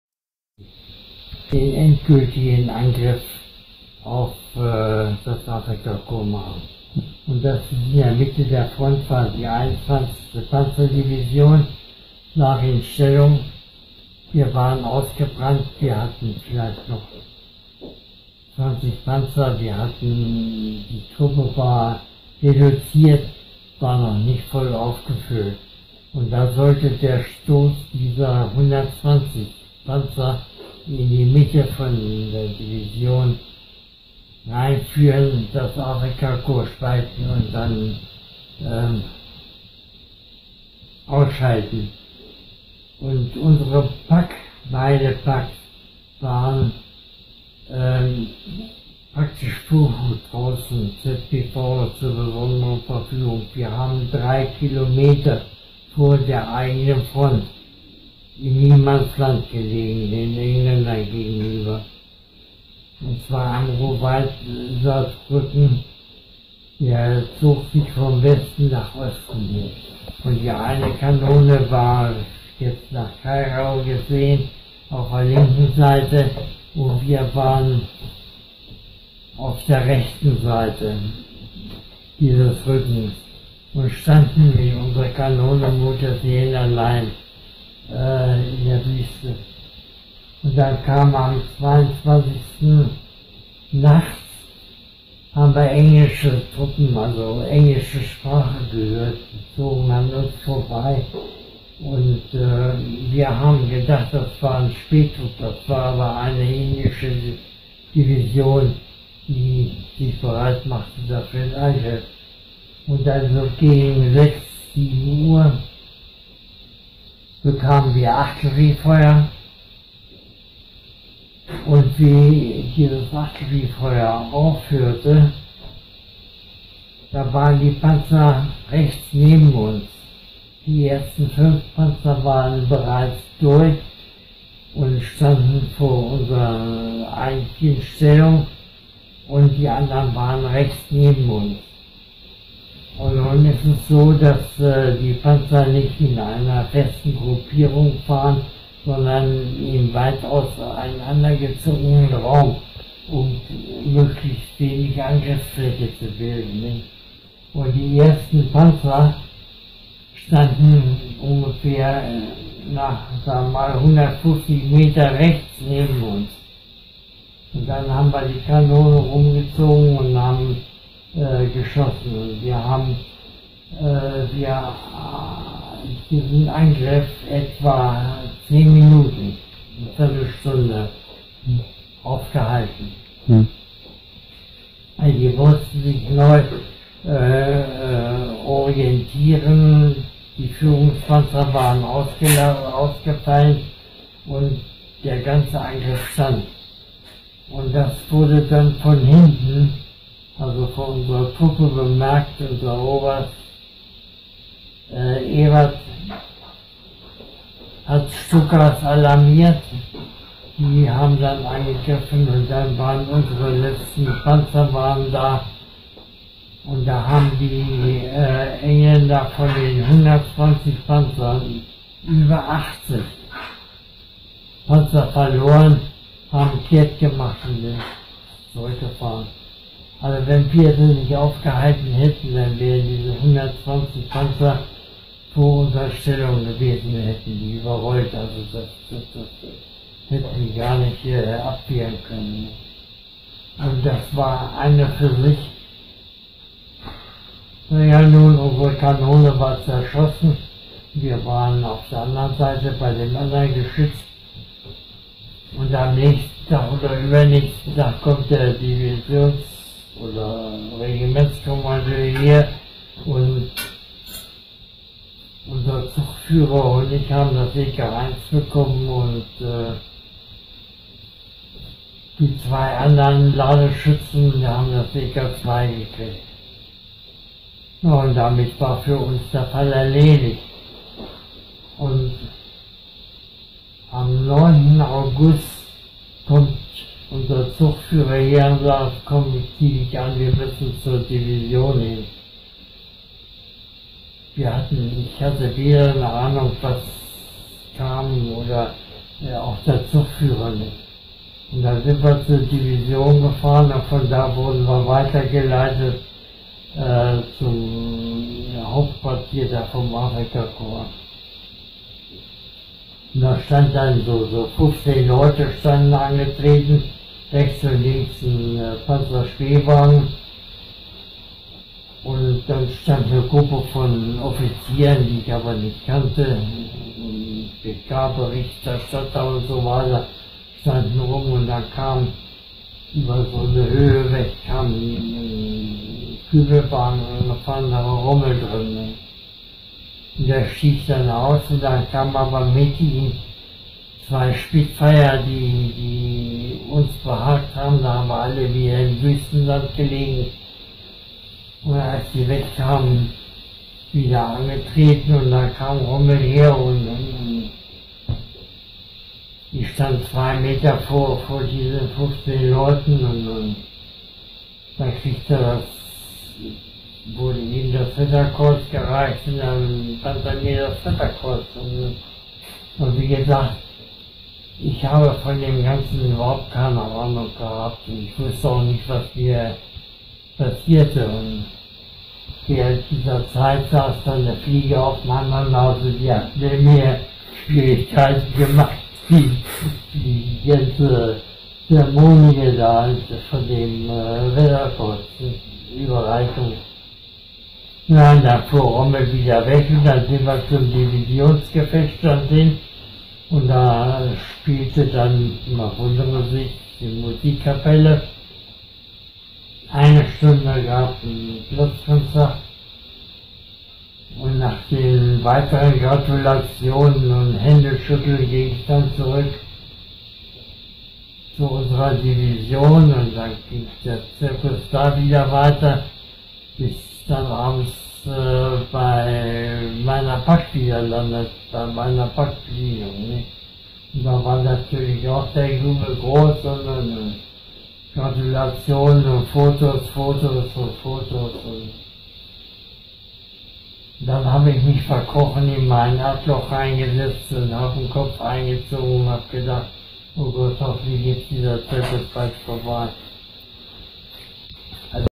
Günter Halm berichtet über seine Kämpfe und Erwin Rommel
Günter Halm berichtet über die Kämpfe in Afrika und wie er Erwin Rommel traf